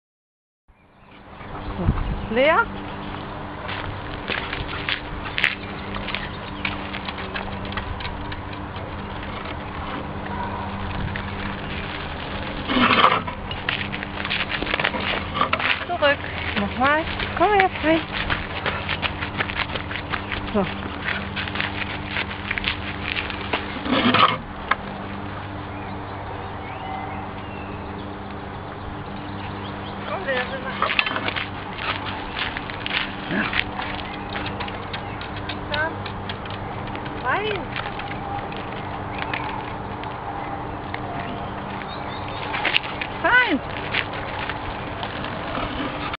Hund balanciert auf dem Brückengeländer
unser Hund Leia Organa Skywalker auf dem Brückengeländer...sie war früher ne Katze sacht man